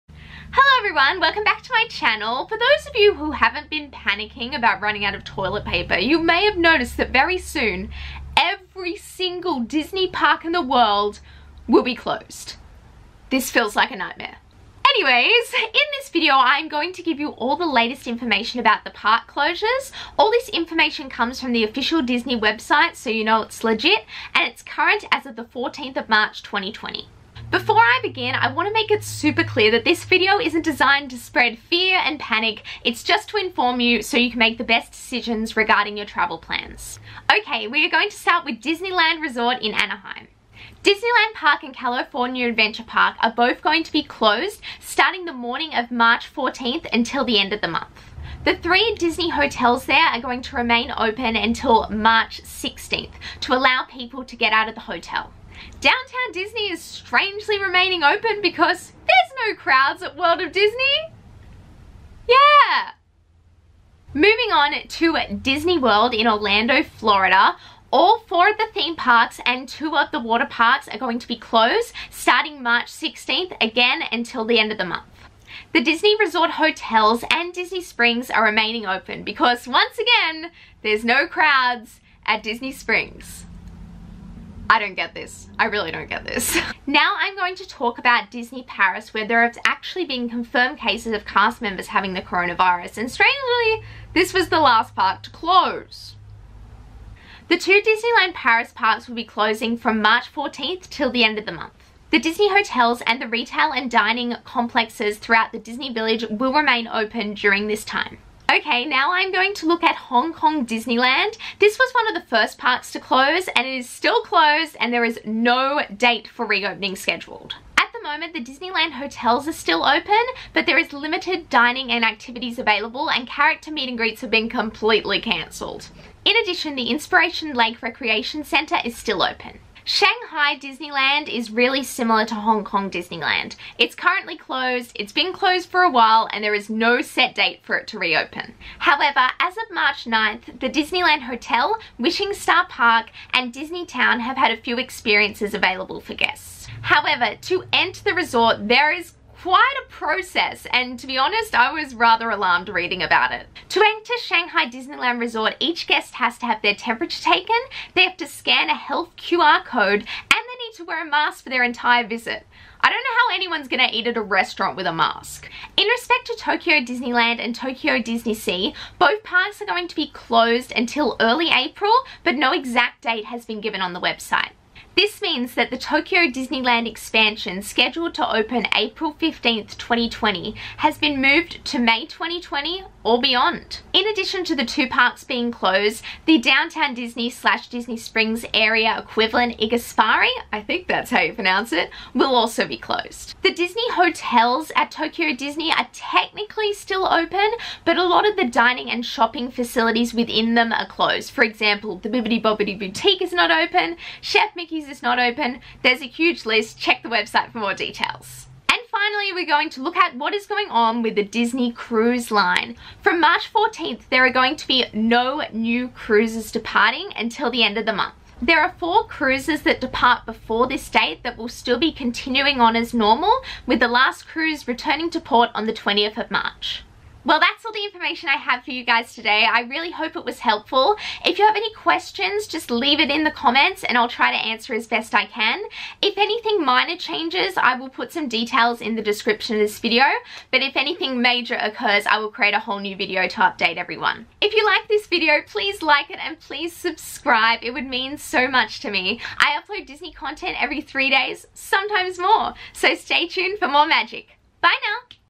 Disneyland and Disney World shut down due to Coronavirus I Important information
Here is the current situation regarding Disney theme parks closing due to the Coronavirus. Hopefully this information is useful and helps you plan your travel for the future.

UPDATES
1. Disney College Program Cast Members from Walt Disney World are required to depart housing on the 18th of March at 11am. Sadly their program will be ending early for them which is devastating.
2. Disney College Program Cast Members from Disneyland will also have their program finishing early.
3. The Disney Village in Disneyland Paris will now close on the 15th of March. The hotels will be closed once the new guests check out.
4. There will be no new Disney cruise departures until April 12th.
5. Disney stores across North America will be closing March 17th. shopDisney.com will still be available.
6. Disney owned locations in Disney Springs in Orlando and Downtown Disney in Anaheim will close beginning March 17th
7. Walt Disney World Disney Hotels will close at 5pm on March 20th.
8. Disneyland and Disney World are now closed indefinitely and will not open on the 1st of April as originally planned.